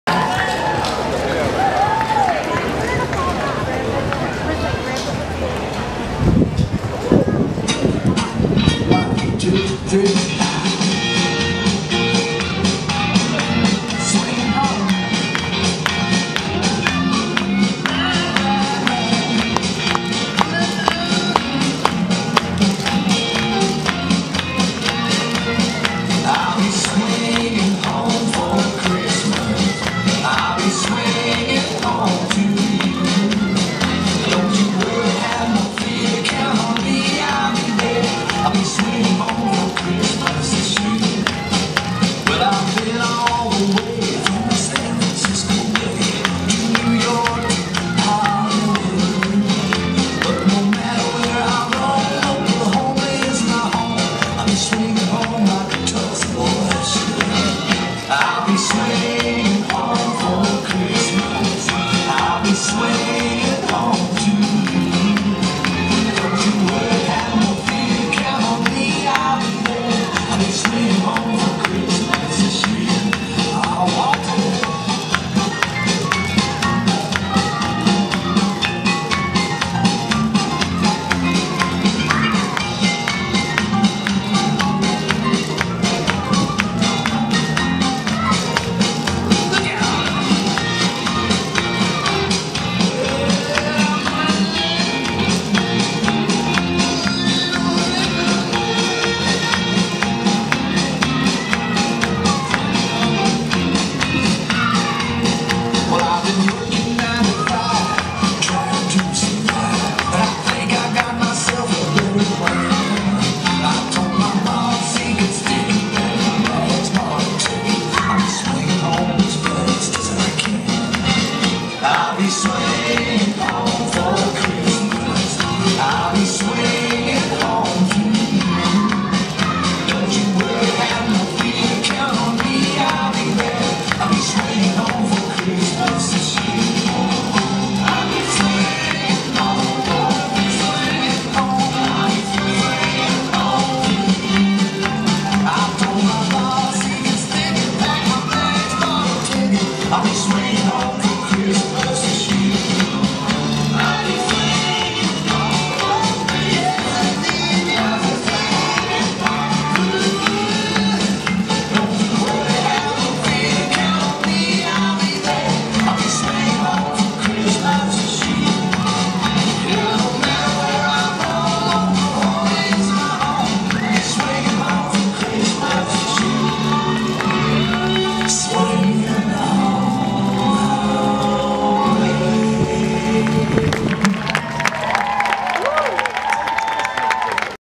Evolution Hoedown at Disney 2016
Here is our PDD Evolution team performing their hoedown for Christmas at Disney Springs in Orlando!